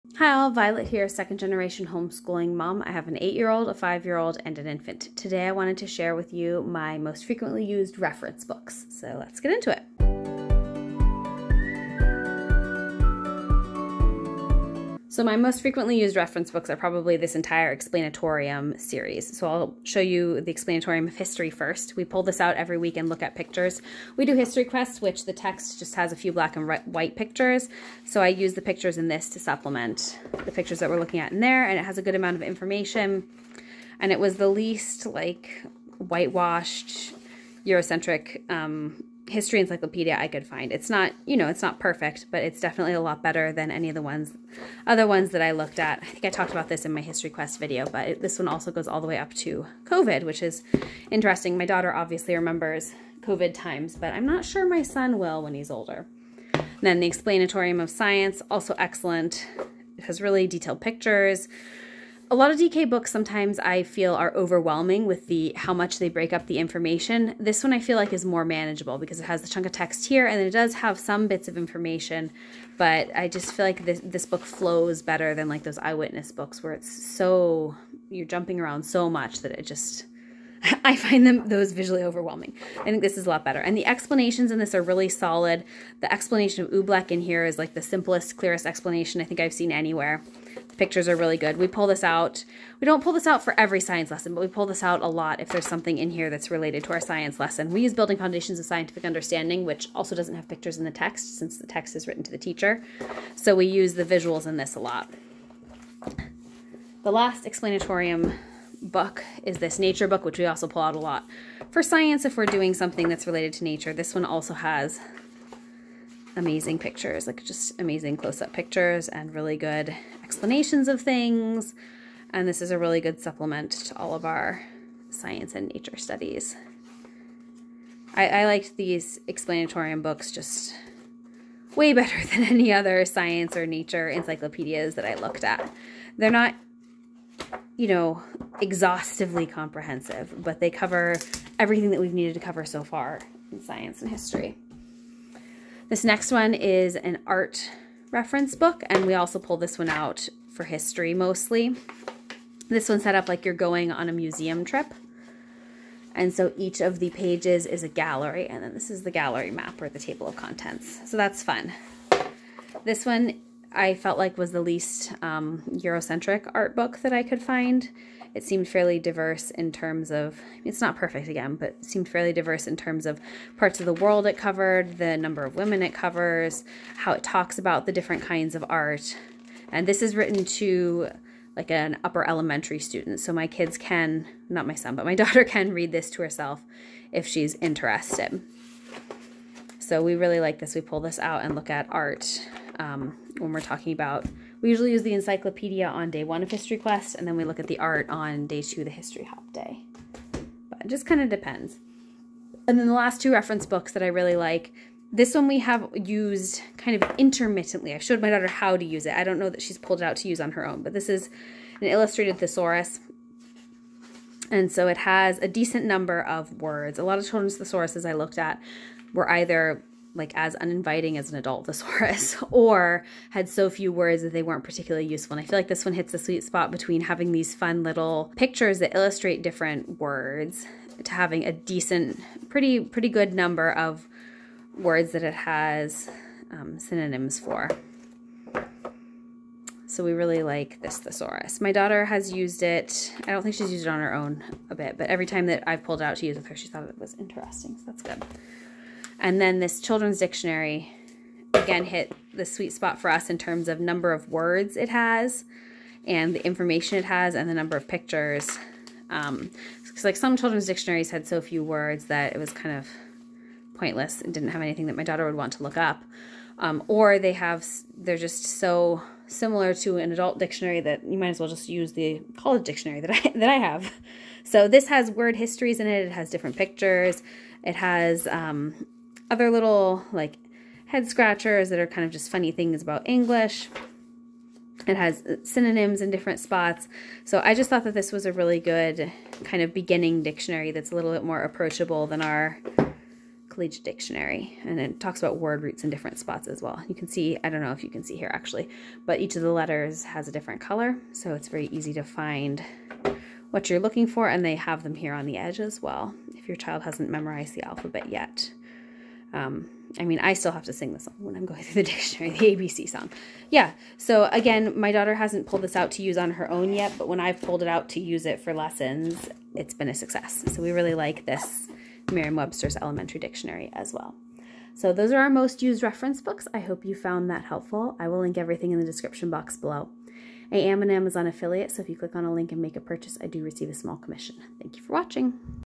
Our Most-Used Reference Books! | Elementary Encyclopedia | Secular Homeschool Resource Flip-through
Hi, all! Today, I wanted to share with you our most-used reference books, including history and science encyclopedias, a children's thesaurus, an elementary dictionary, and an art reference book. I hope you find this helpful!

Links:
Explanatorium of History
Explanatorium of Science
Explanatorium of Nature
Ultimate Art Museum
Children's Illustrated Thesaurus
Elementary Dictionary